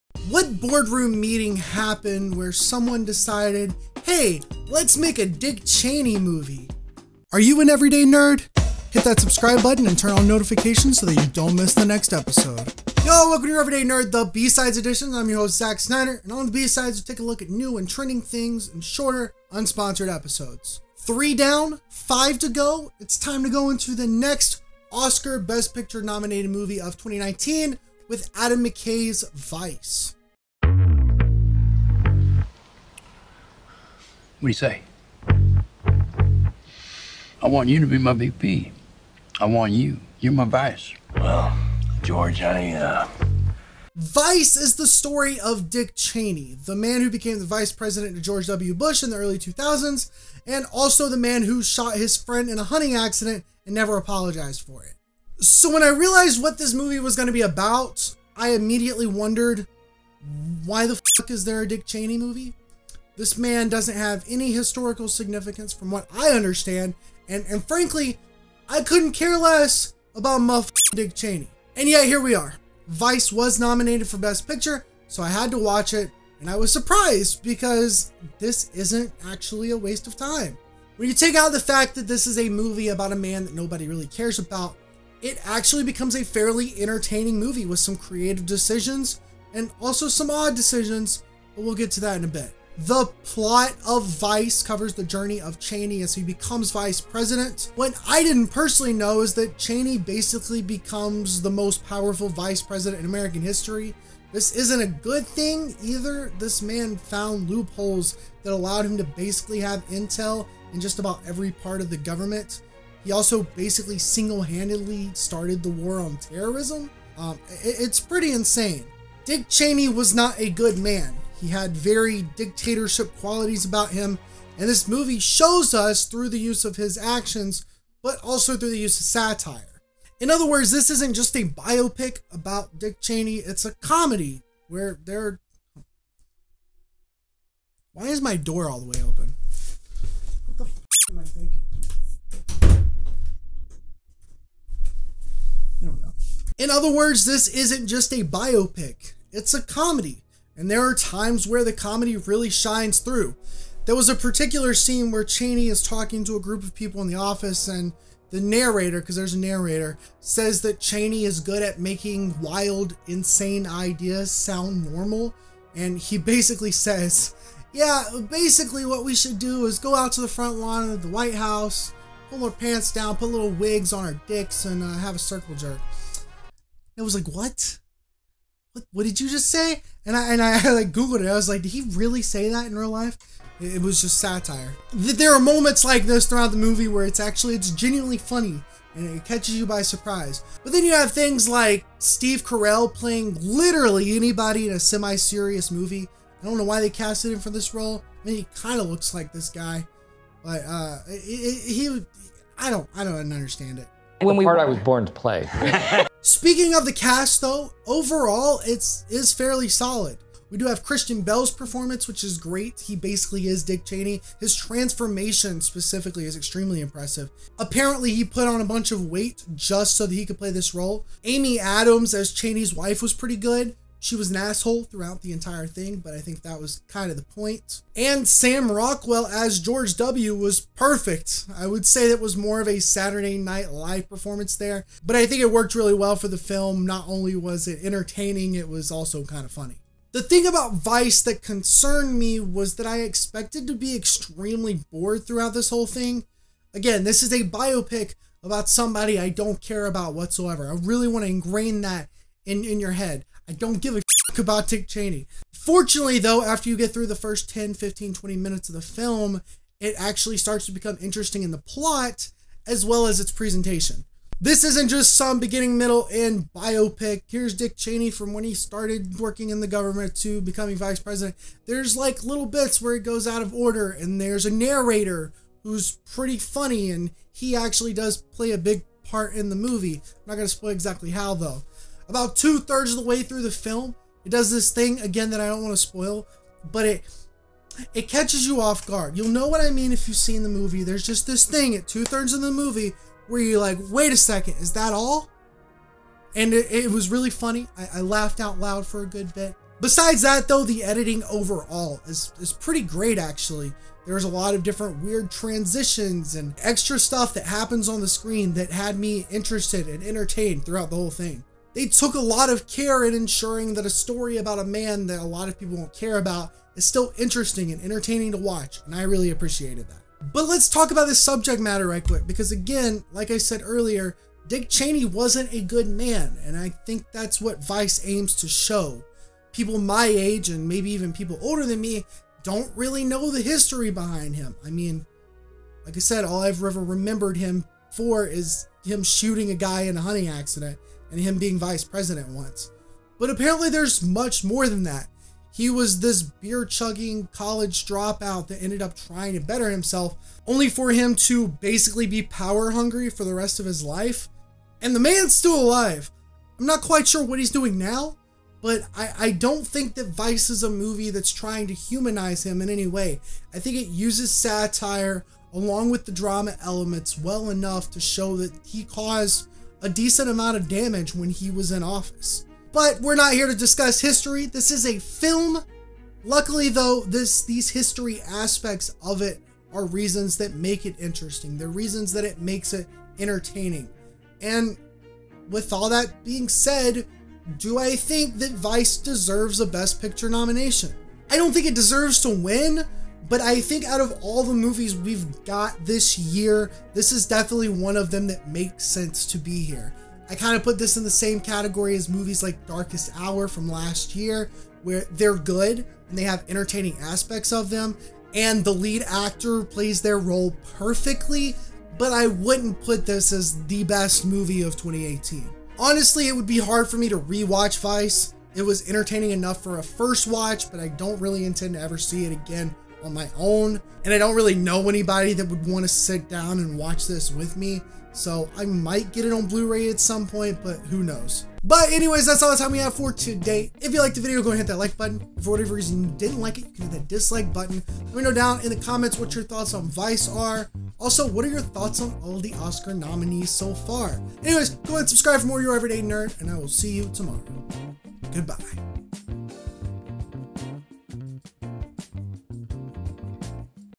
Does Vice Deserve Best Picture 2019? | YOUR EVERYDAY NERD
YEN #063 - Vice is directed by Adam McKay and stars Christian Bale, Amy Adams, and Sam Rockwell. Vice was nominated for Best Picture for the 2019 Oscars, but does it deserve it?

Thank you to my Patrons! ► CaffeineDragon, Cowfro

Go check out the merch!